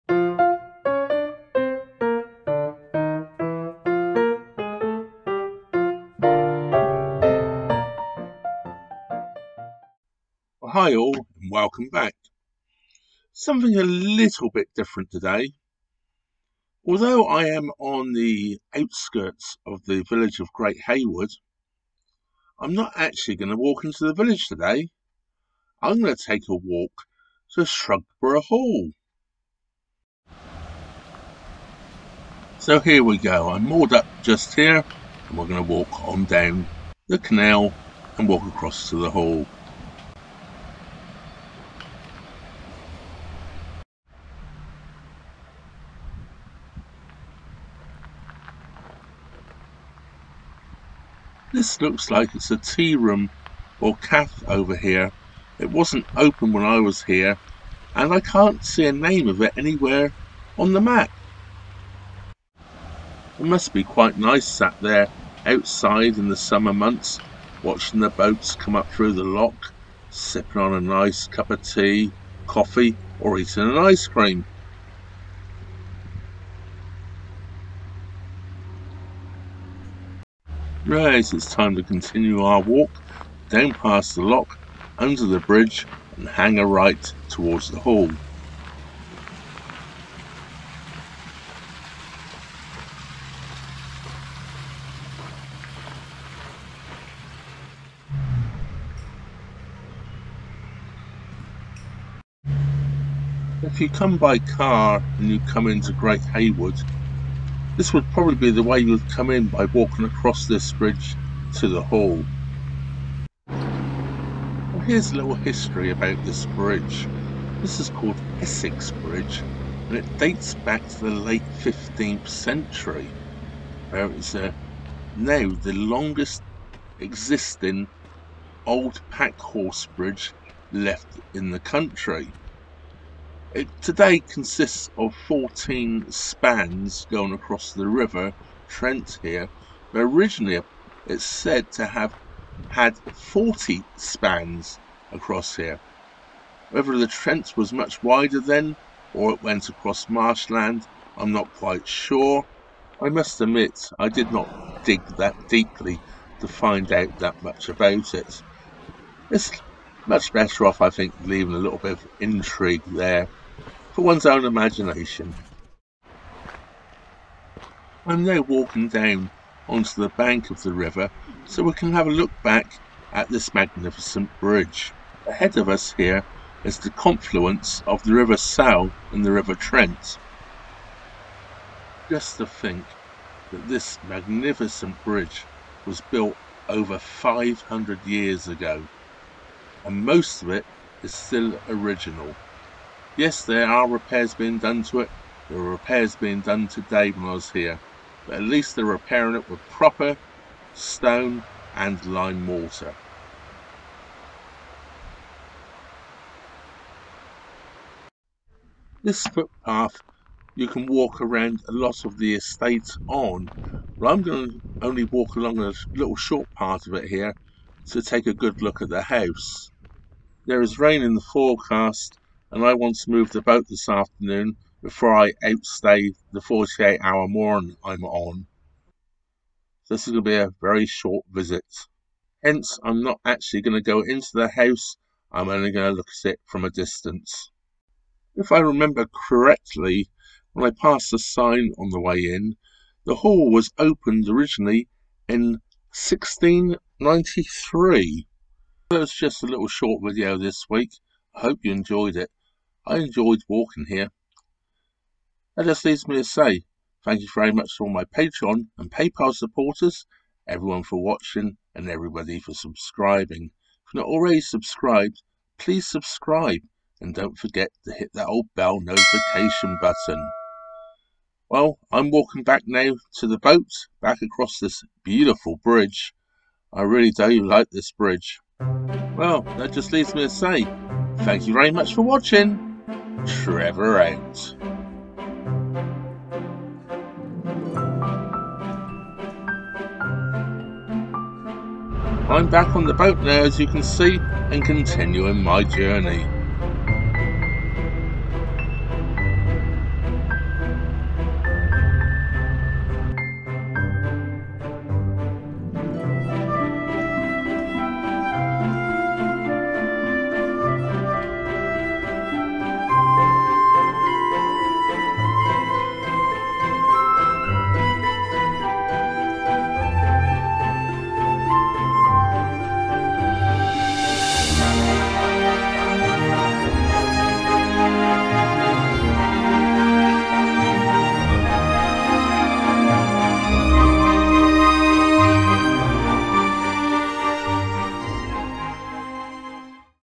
Essex Bridge to Shugborough Hall
I might be moored next to the village of Great Hayward, but I make the decision to walk in the opposite direction, across the grade I listed packhorse bridge, Essex Bridge, and take a look at Shugborough Hall.